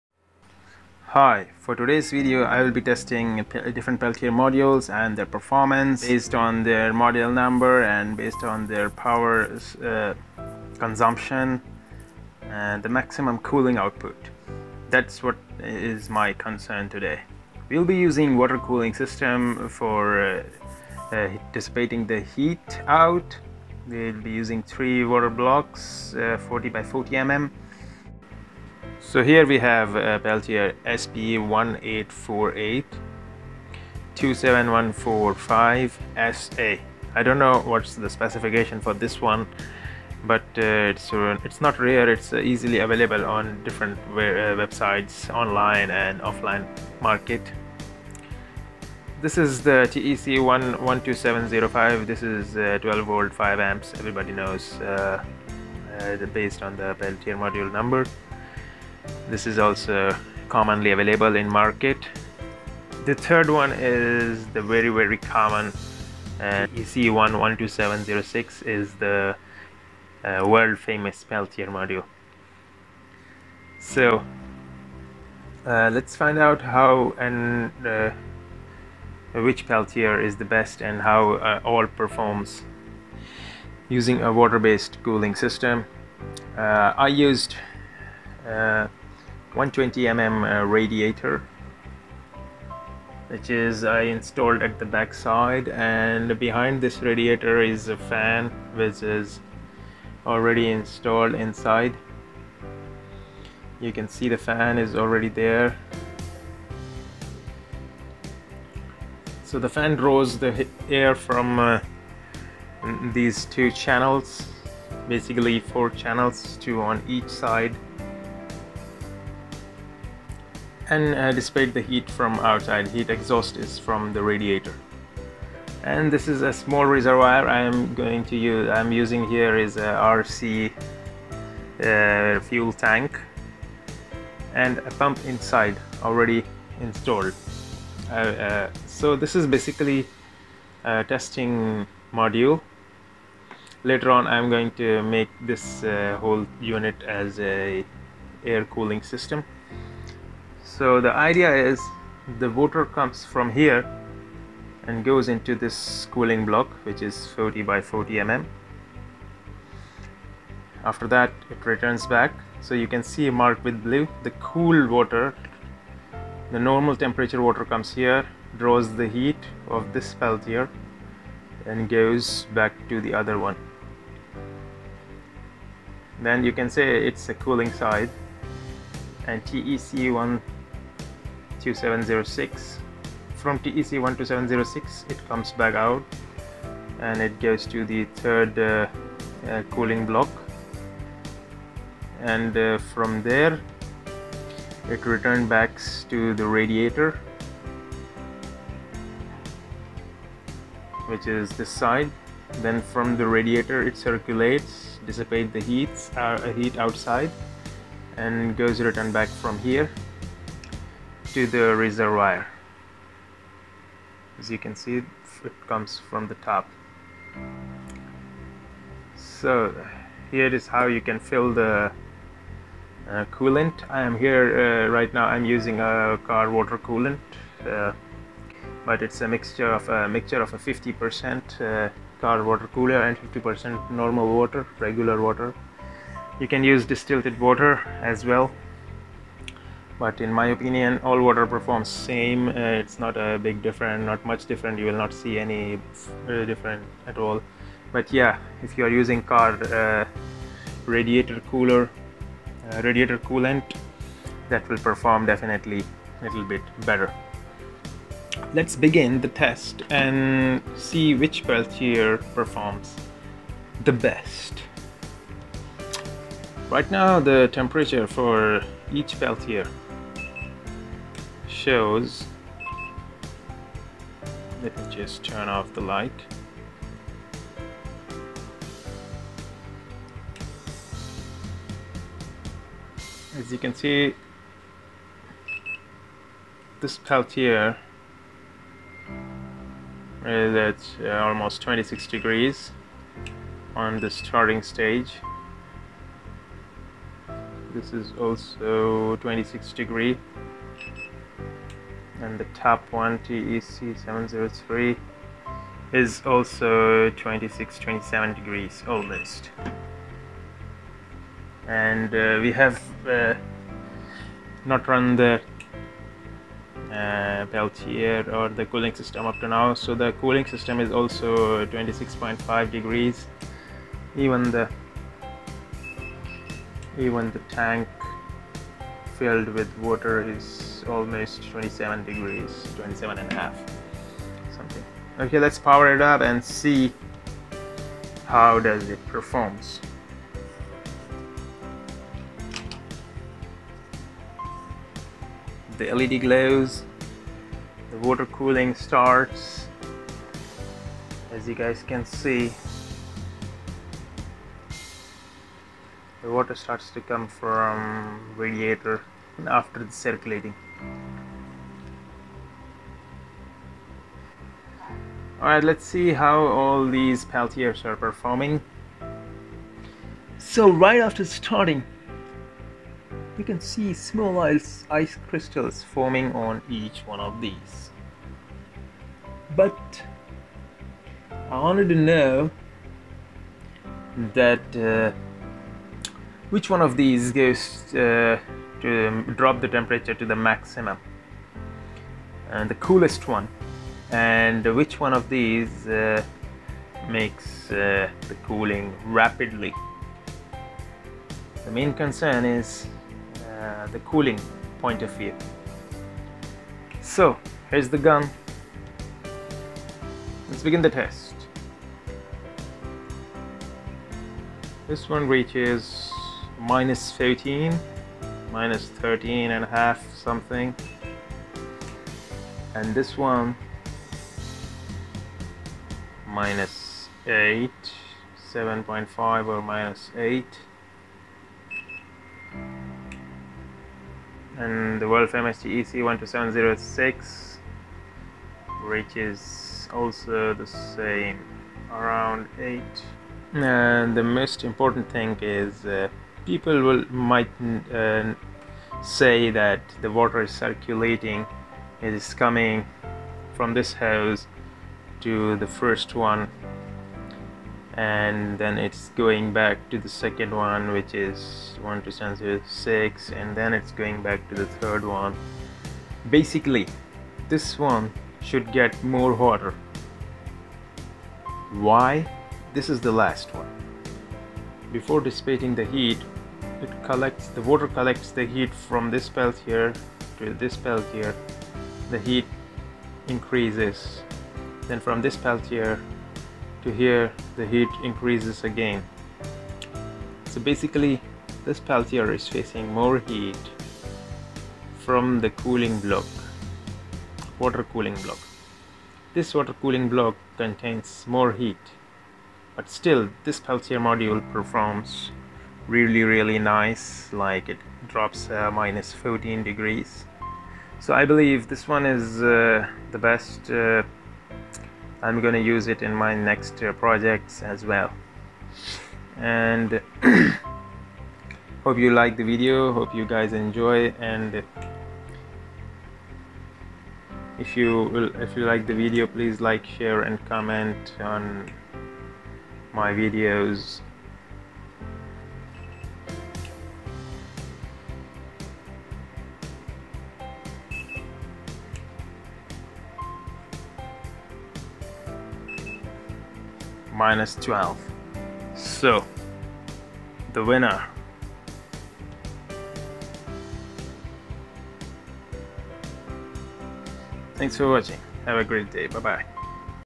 Peltier vs peltier vs peltier Using water cooling radiators and coolant
In this video I will be performing a test between different peltier modules Using water cooling radiators and coolant.. To see which one of 3 performs the best..

The results are based on non contact infrared thermometer.

And the end result is my personal opinion.

Item list

Different peltier modules
40x40 mm water cooling blocks
120mm radiator
120mm brushless DC 12v fan
12v brushless water pump
Hose for connecting all system
Car radiator coolant.

Mere dosrey videos b zarur dekhen..

DC BRUSHED MOTOR UPGRADING